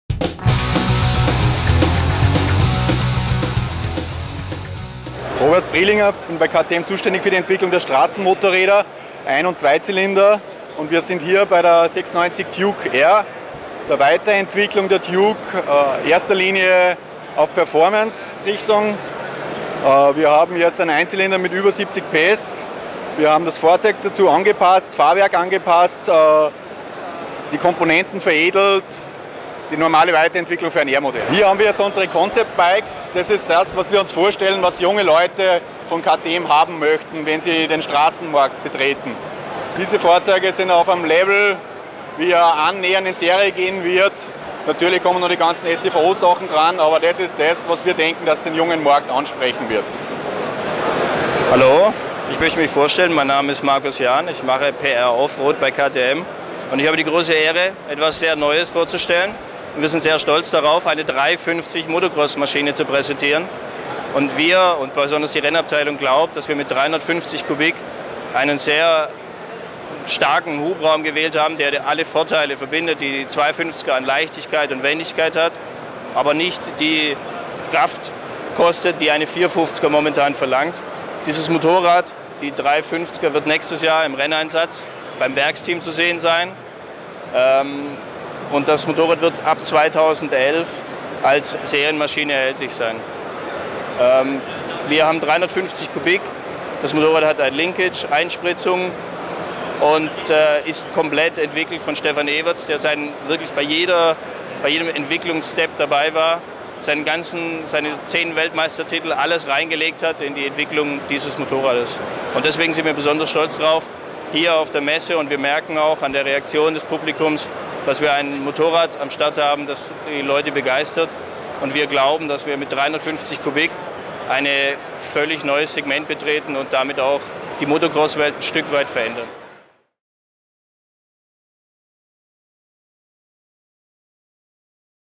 EICMA 2009 - KTM news
1000PS shows the new KTM models presented at the EICMA 2009.
1000PS zeigt die neuen KTM Modelle auf der EICMA 2009.